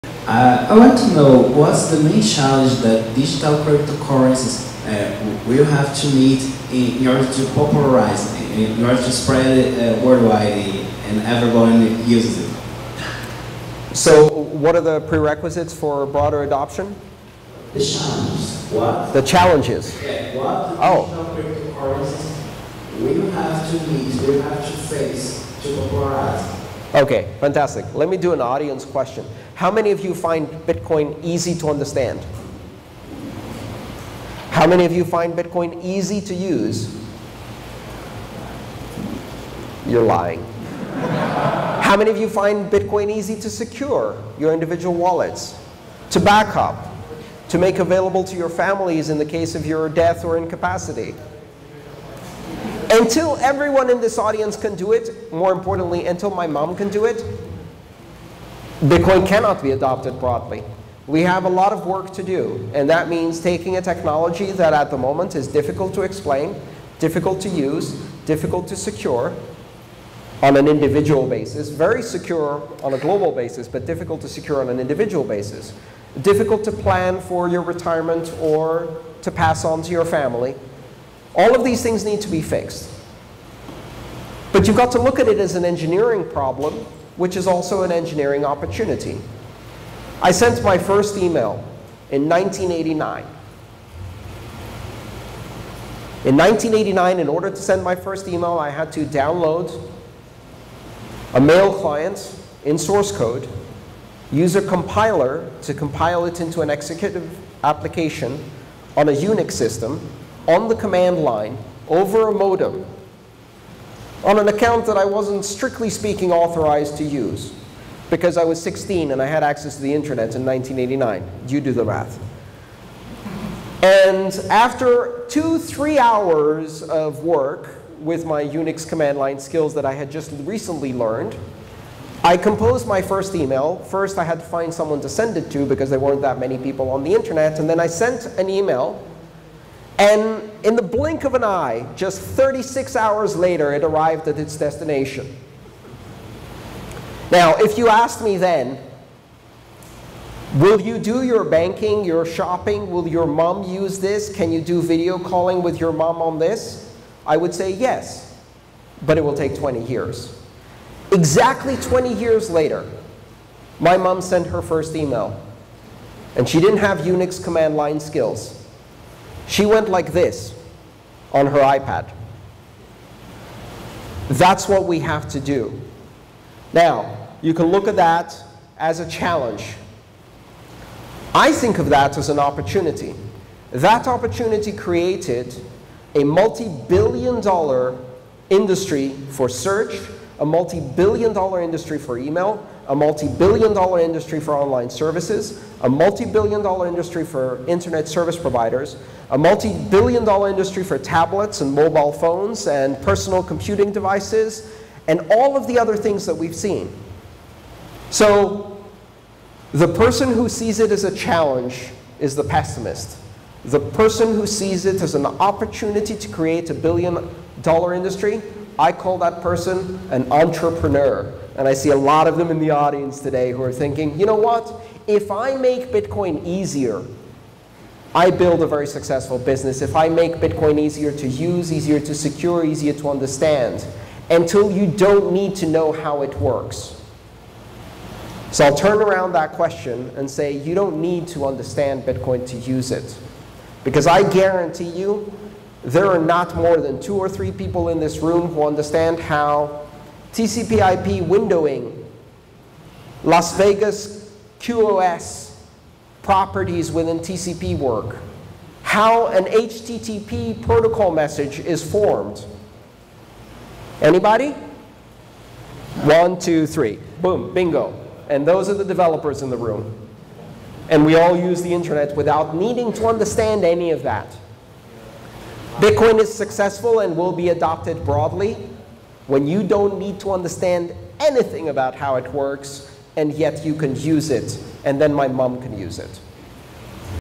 Bitcoin Q&A: What Are Challenges to Achieving Mass-Adoption?
Chapters
0:00 What is the main challenge that cryptocurrencies will have to meet in order to popularize and spread it worldwide, where everyone uses it?
1:01 We have a lot of work to do
4:35 Bitcoin will be successful and adopted broadly when you don't need to understand anything about how it works